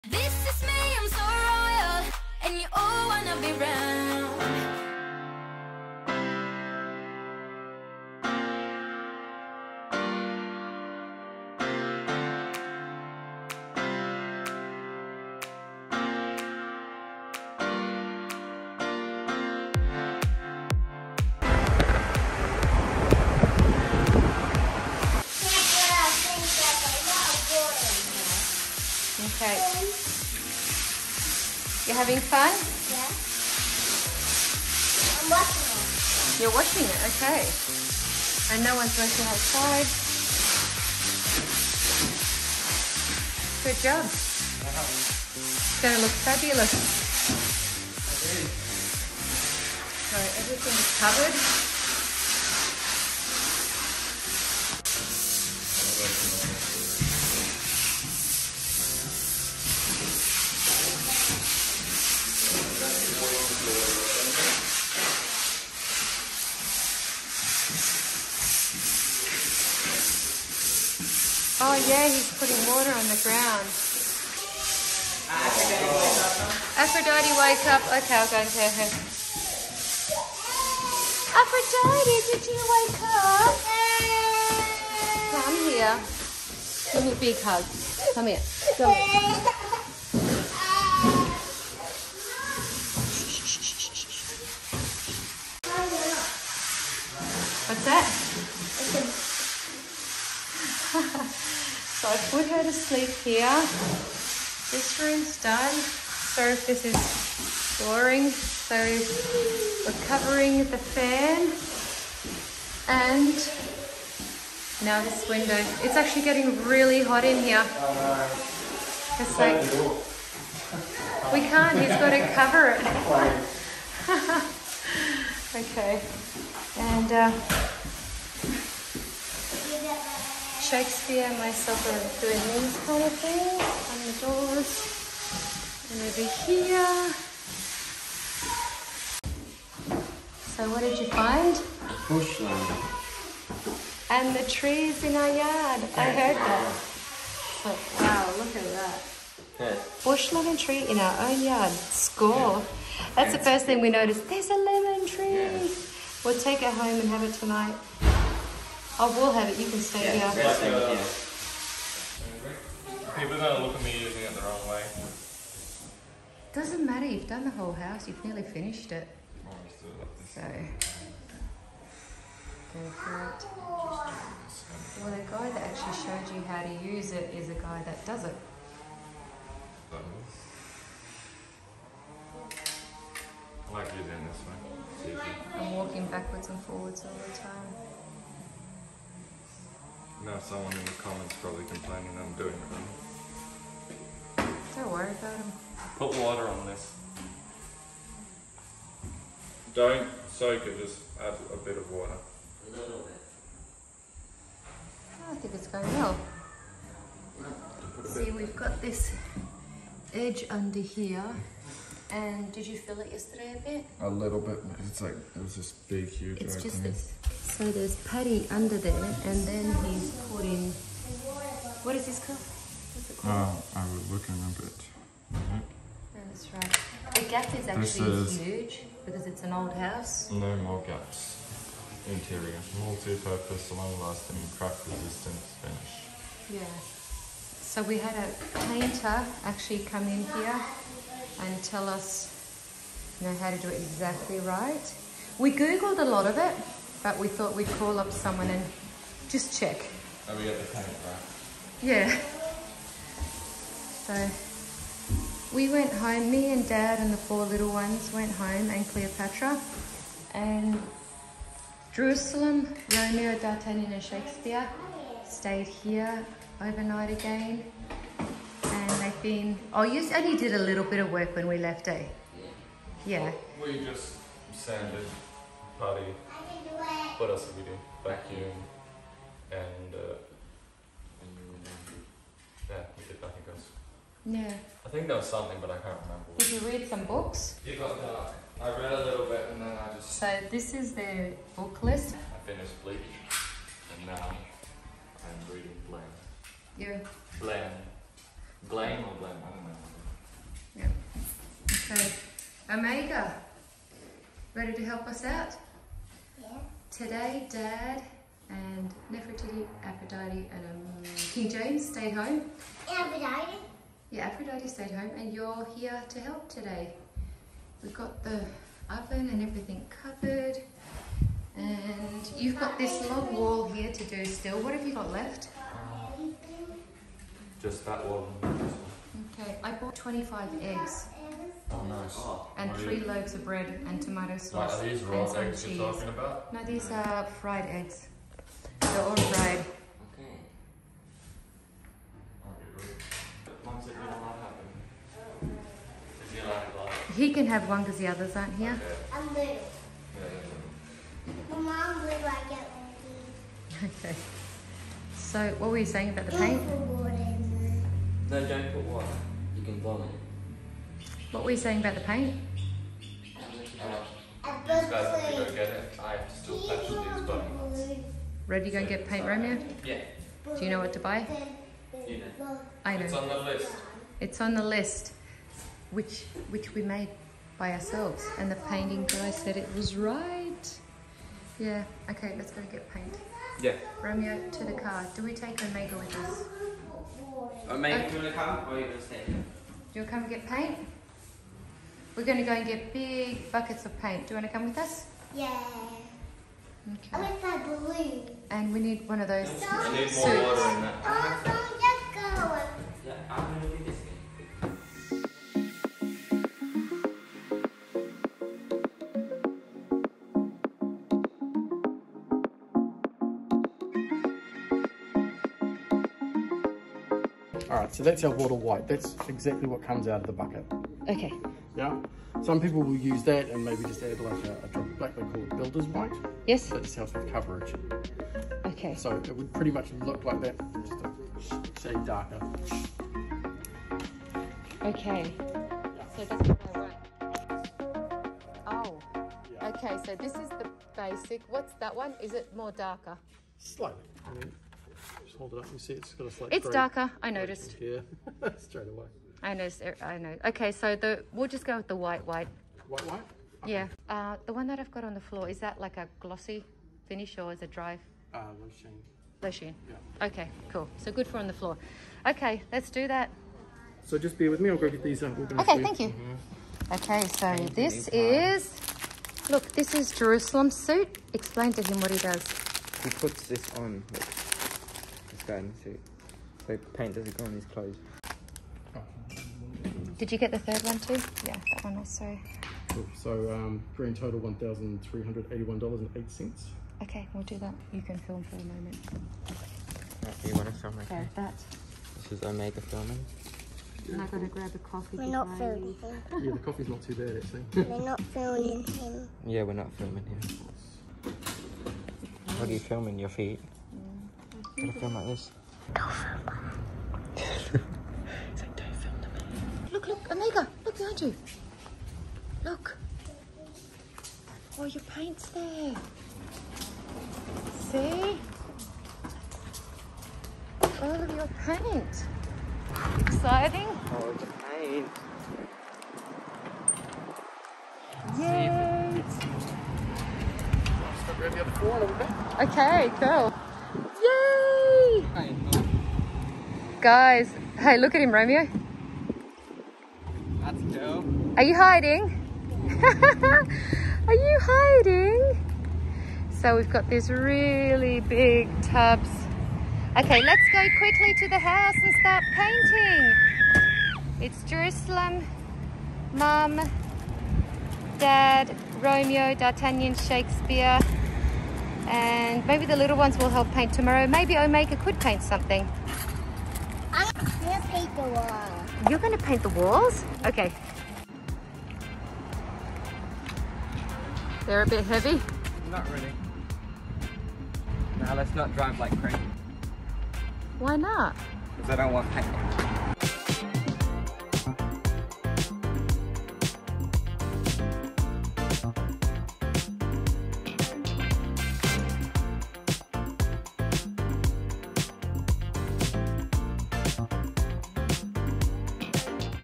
What colour should we paint the floors? / PAINT SHOPPING / The Mom of 10 /
It's so controversial! SHOULD or SHOULDN'T I paint the timber floors?. If I do. WHAT COLOUR? White, Blue, Black, Grey. ? Come paint shopping with The Mom of Ten. We need to make a decision FAST! Cleopatra is sleeping again! LOL.